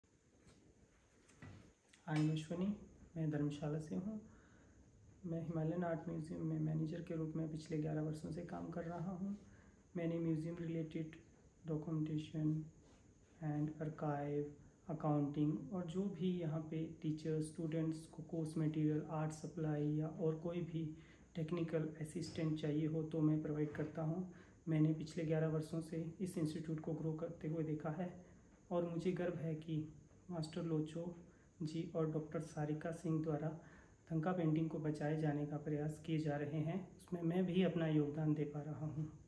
Manager at Centre for Living Buddhist Art - Dharamshala
'Center for Living Buddhist Art’ was co-founded by Master Locho and Dr. Sarika Singh, Finest Masters of Buddhist Paintings. The Center includes‘ Thangde Gatsal Art School and Studio’ and ‘Himalayan Art Museum’

Ashwani Kumar is a administartion and technical manager at the Centre for Living Buddhist Art for over a decade.